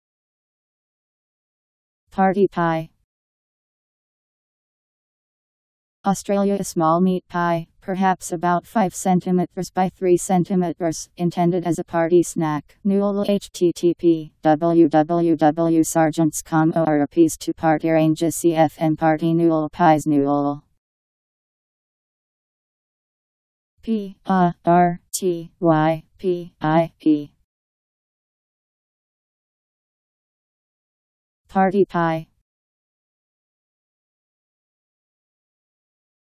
Party pie Meaning
Video shows what party pie means. (Australia) A small meat pie, perhaps about 5 centimetres by 3 centimetres, intended as a party snack. Party pie Meaning. How to pronounce, definition audio dictionary. How to say party pie. Powered by MaryTTS, Wiktionary